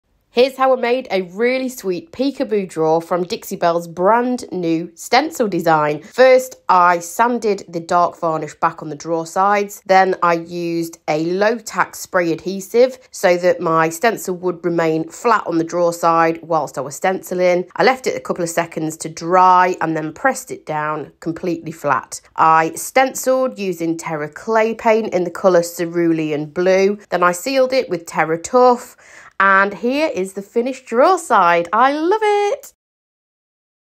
How-To Stencil The Drawer Sides Of Your Furniture With No Hassle!
In this Connie from Faff Designs is going to show us an effortless way of applying a stenciled design to the drawer sides of her furniture, featuring the new "When Life Gives You Lemons" Silkscreen Stencil from the Belles and Whistles line at Dixie Belle Paint! These will be available for purchase on the website starting March 1, 2023. BUT, to get your hands on them now, find a local retailer near you